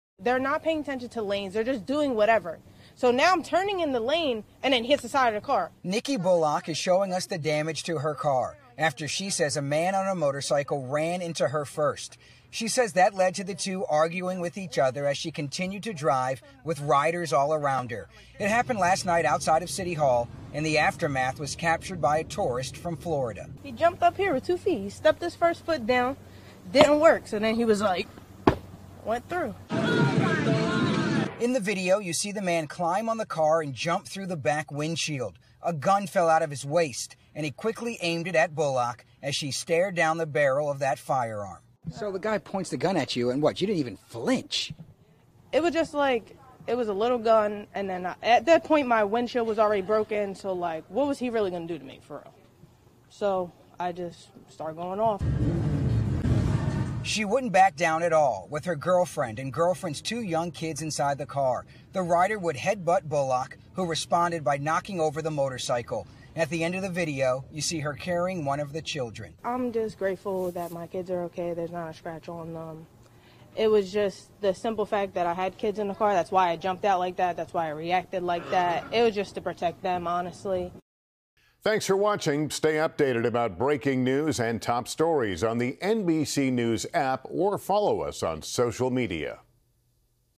Viral video shows biker smash windshield, then pull gun on driver
Philadelphia police are searching for a motorcyclist seen smashing a car's back windshield and pointing a gun at its driver after a minor accident. WCAU's Aaron Baskerville reports.